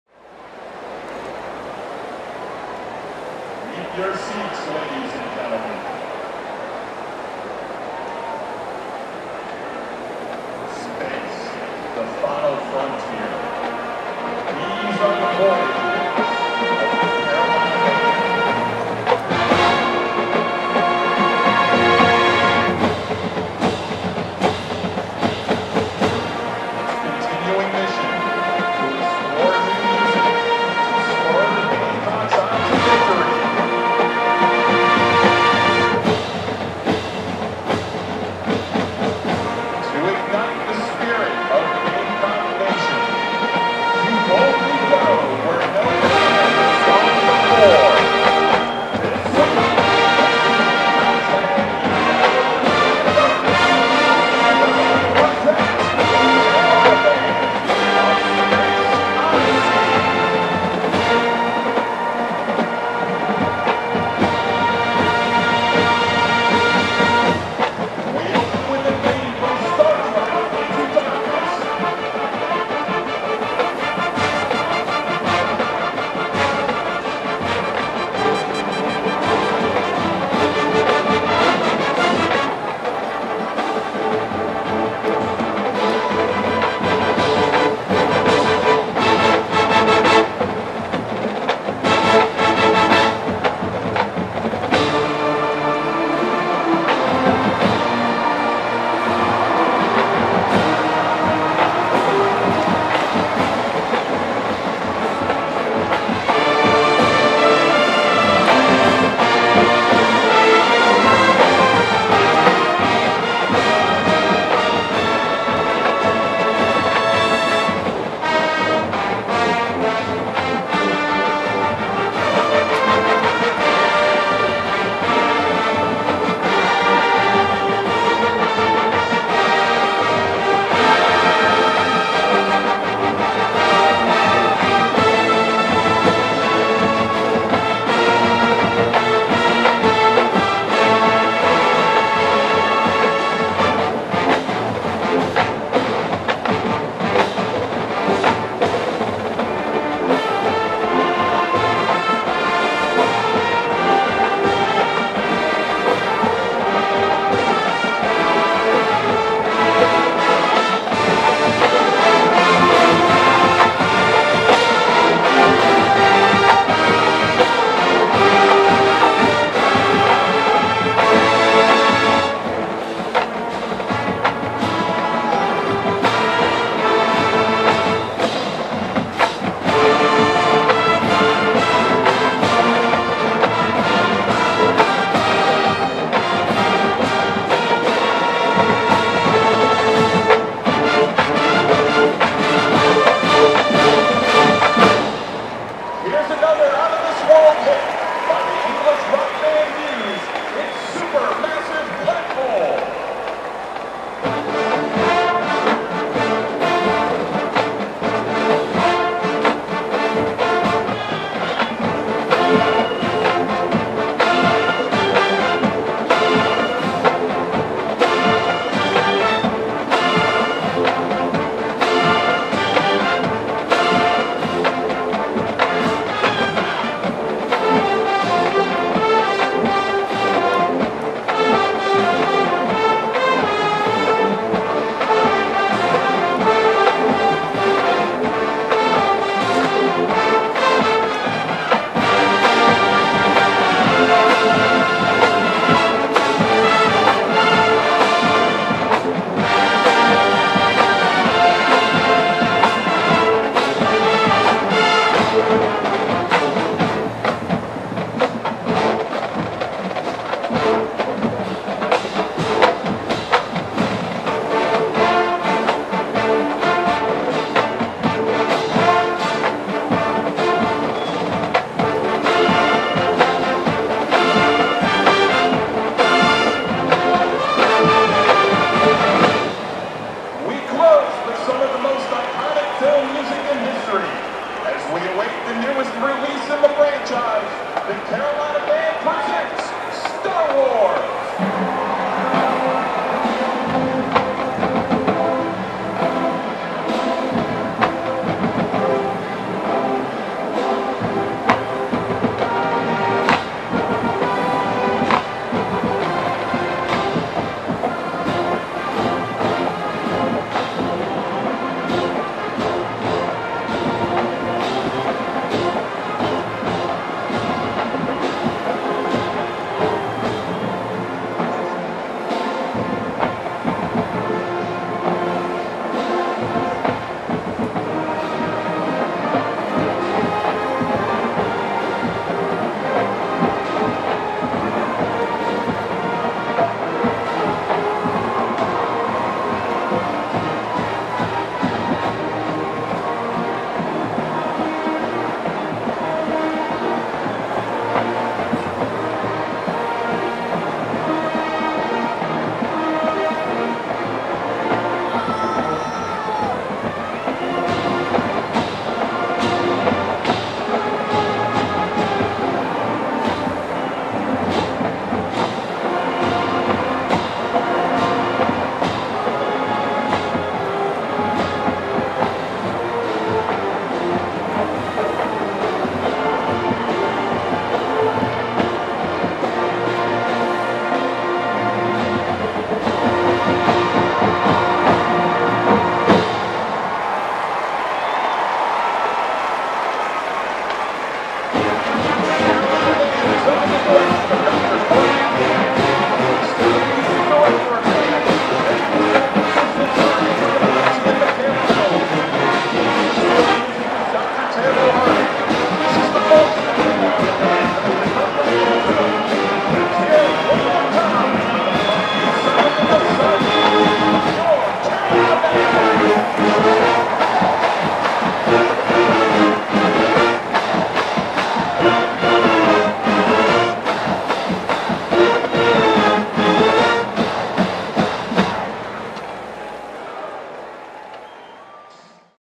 Carolina Band - A Space Odyssey
Halftime show "Carolina Band: A Space Odyssey" Texas A&M Game October 1, 2016